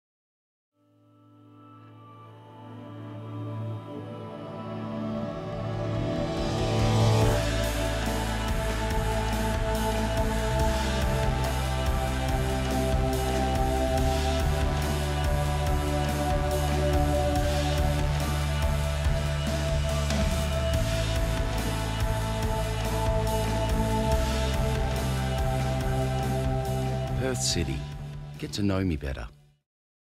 Perth City - Get To Know Me Better 1 HD
I shot and created VFX for the closing "superdolly" shot in this Perth City campaign.

Directed by: Robert Forsyth
Production: The Penguin Empire
Grade and Edit: Merlin Cornish at Siamese
Additional post: Andrew Buckley at Siamese

Recently awarded 'Gold' at the WA Tourism Awards for Destination Marketing.